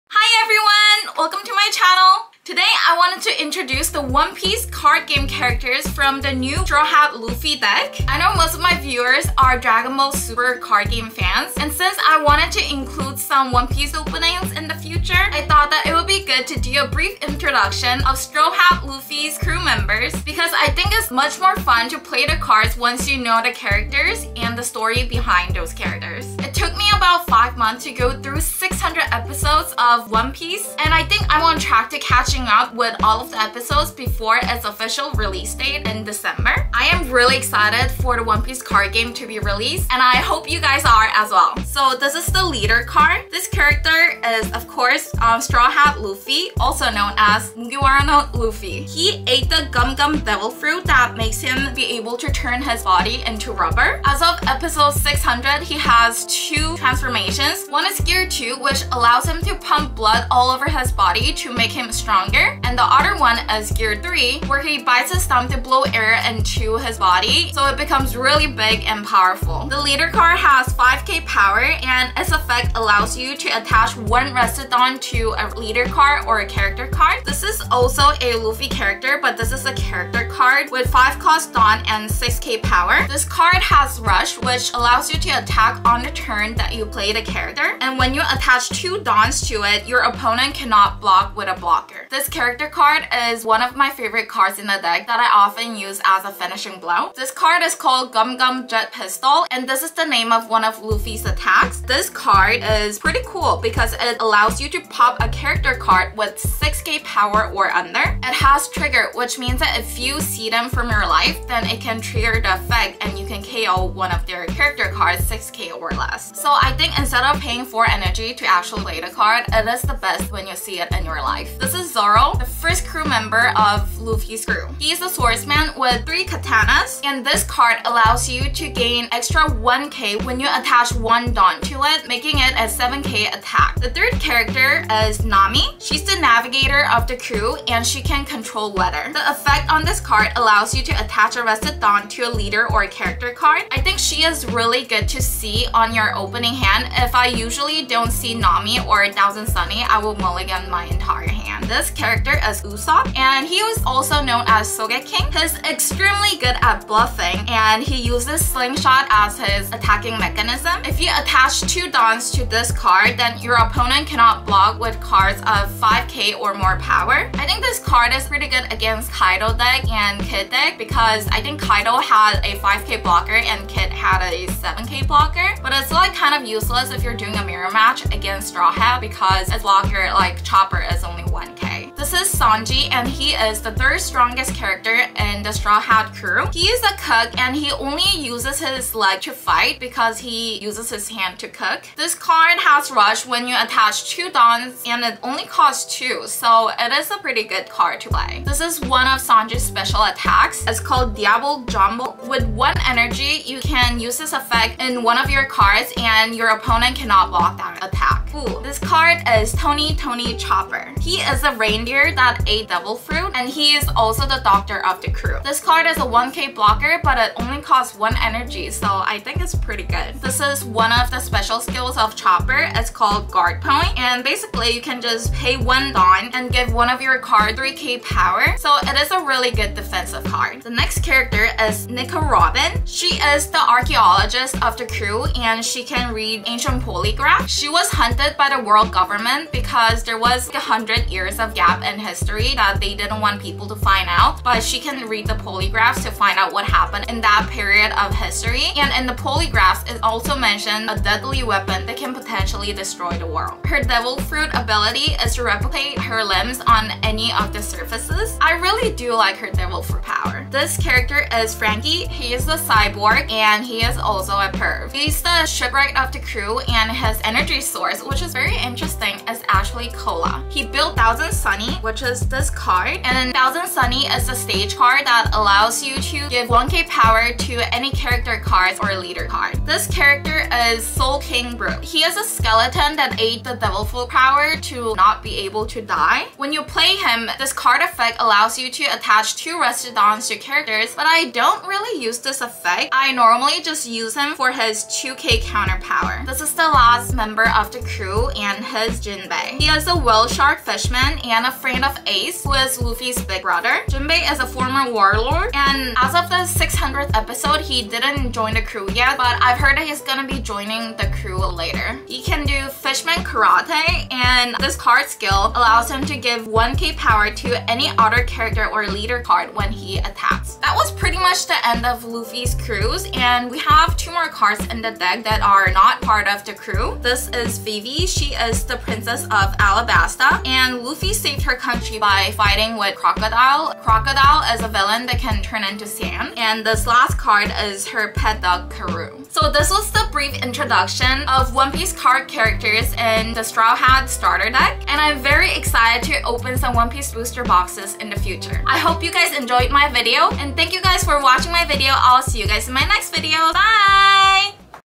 Introducing Straw Hat Luffy Starter Deck Crew Members
This is a brief introduction of the Straw Hat Luffy’s Crew members in the starter deck!! I’m so excited for the English One Piece cards to be released and I hope you guys enjoy my video!

*I think the Usopp card was a mistranslation online and I totally butchered poneglyph 😅